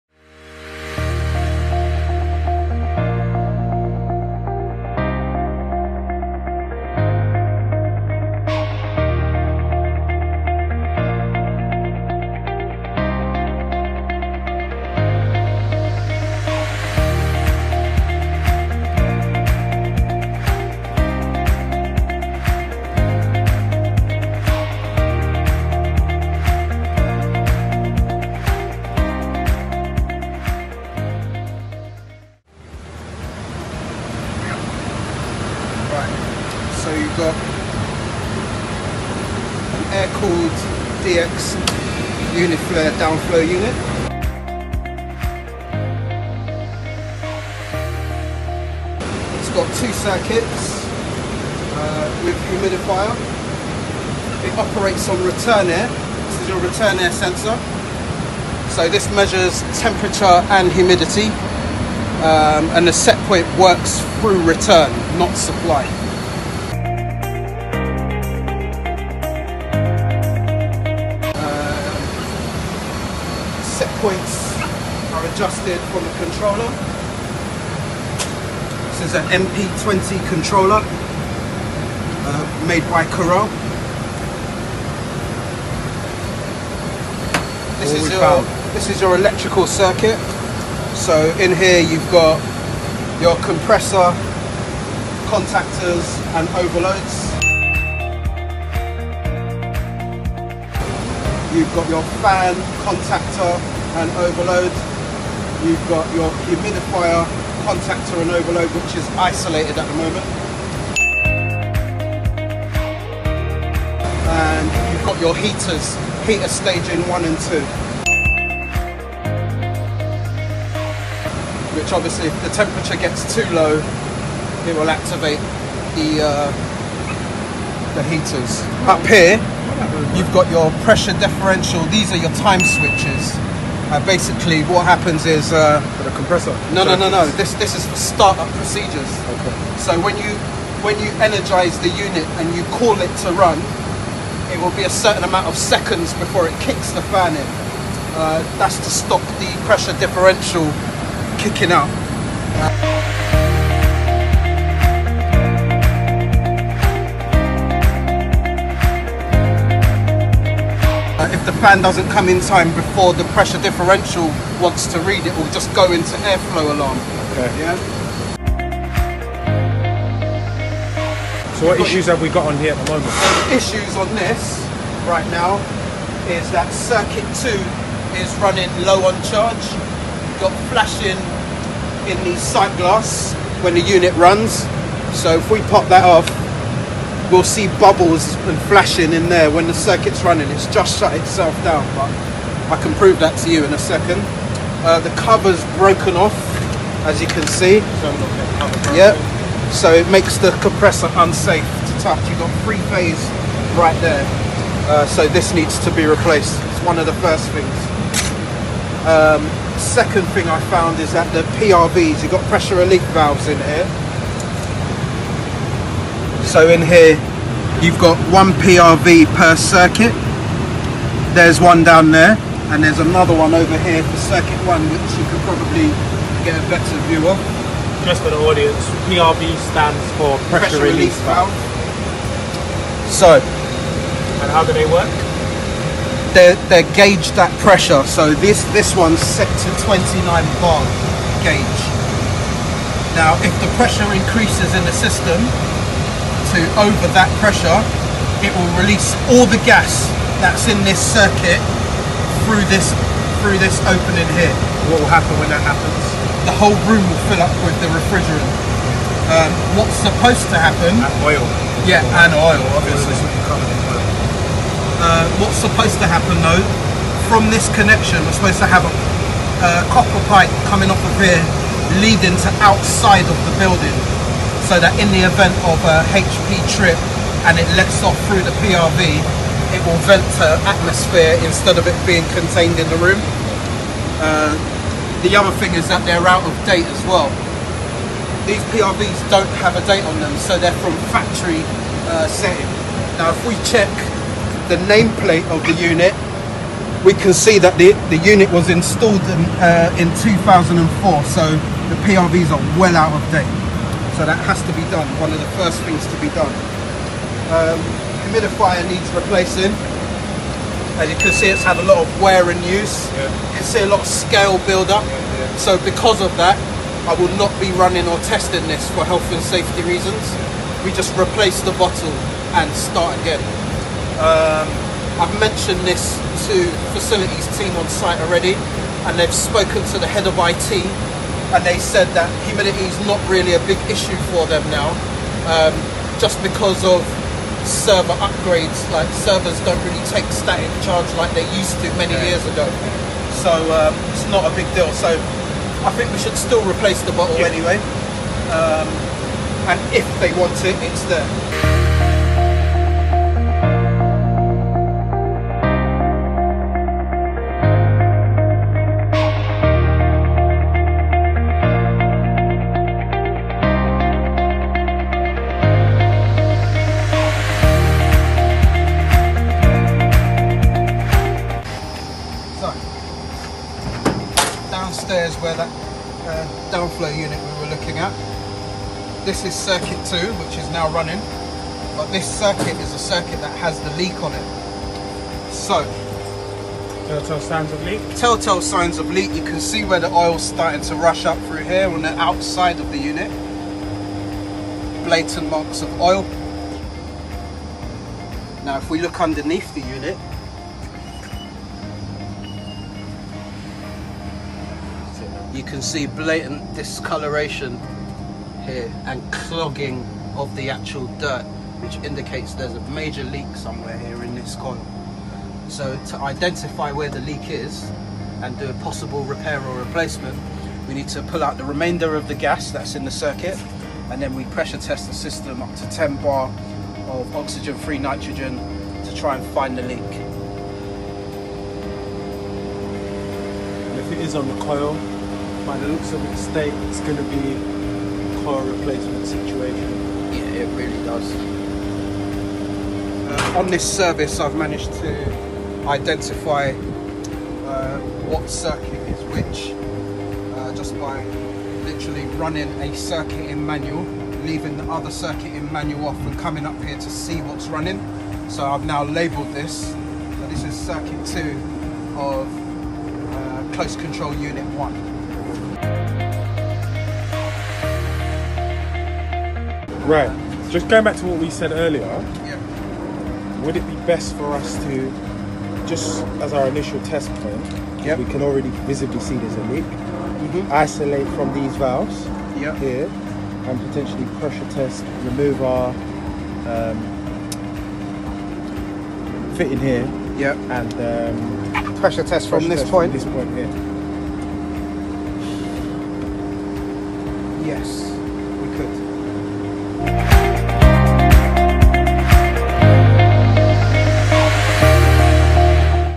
Server Room Air Conditioning How To Video | Eco Climate Solutions
We are here to help.

This recent video shows our Service Manager investigating the 'Uniflair' Downflow Units on one of our commercial sites in London.

These works were performed as part of our Server Room Air Conditioning works. During the process of working through our HVAC design and Consultancy Services for the commercial site,  we identified that the Server Room Air Conditioning had some major issues - including a leak on one of the circuits.

Having identified multiple issues on site, we were then able to report back to our client, offering various remedial options and repair works within the clients budget.